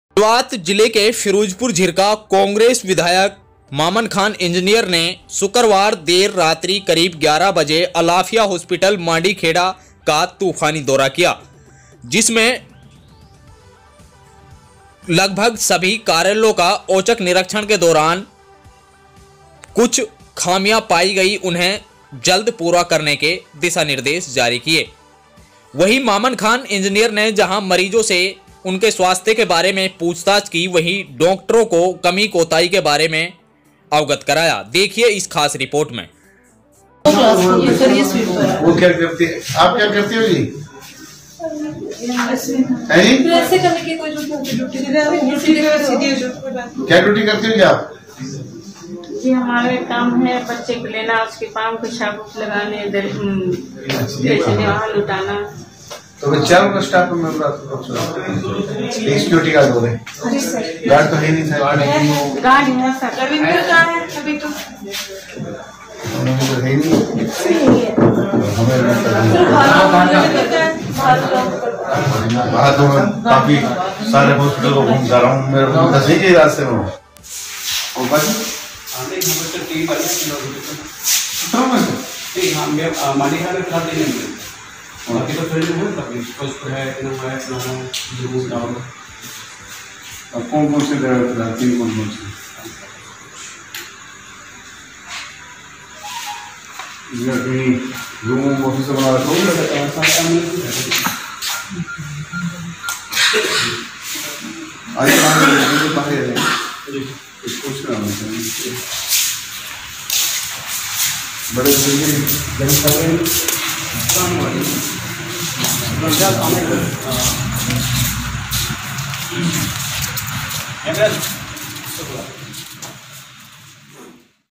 गब्बर निकले मेवात के विधायक मामन खान इंजीनियर, रात्रि 11 बजे किया हॉस्पिटल का दौरा,लिया जायजा।
टुडे आजतक मीडिया न्यूज़ नेटवर्क पर बिलाल अहमद पत्रकार की खास रिपोर्ट।